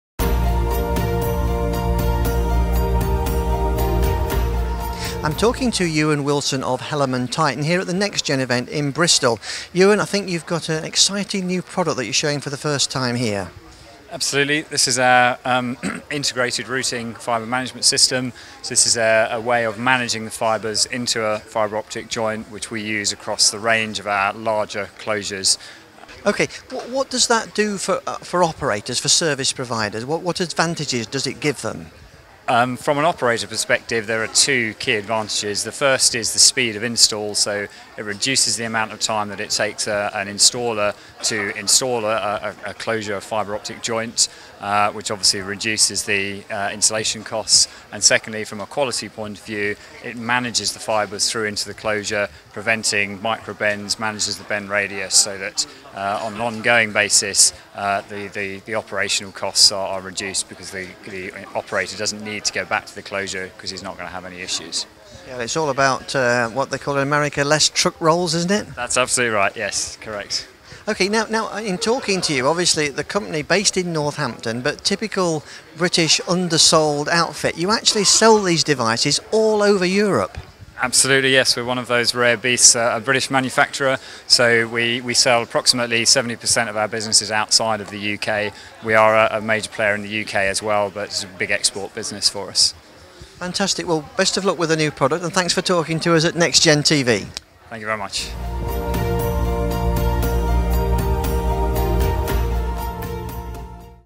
Ewan Wilson - MD HellermannTyton
Ewan Wilson - MD HellermannTyton speaks to NextGen TV at the NextGen South West and Bristol Roadshow on 17 July 2012